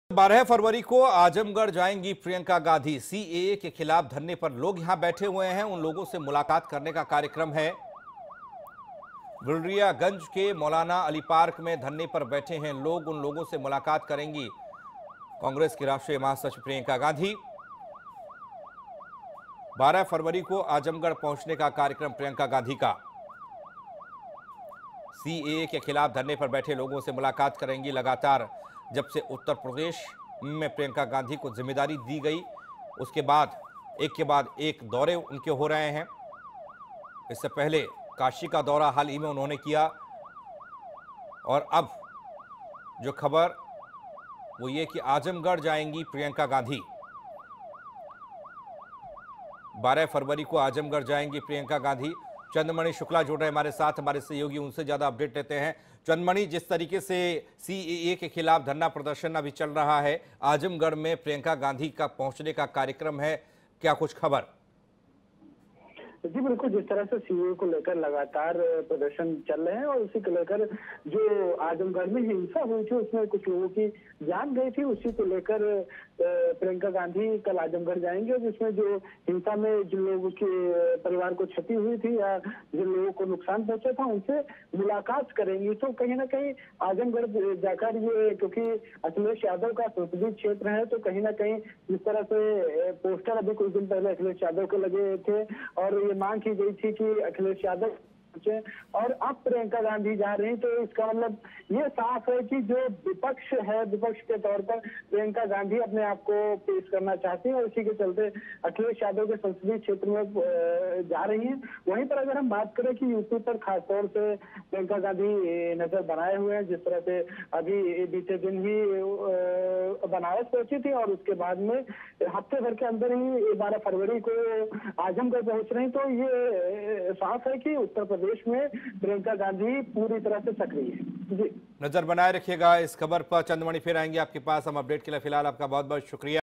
प्रियंका गांधी 12 फरवरी को करेंगी आजमगढ़ का दौरा
12 फरवरी को आजमगढ़ जाएंगी प्रियंका गांधी
CAA के खिलाफ धरने पर बैठे लोगों से करेंगी मुलाकात
बिलरियागंज के मौलाना अली पार्क में धरने पर बैठे हैं लोग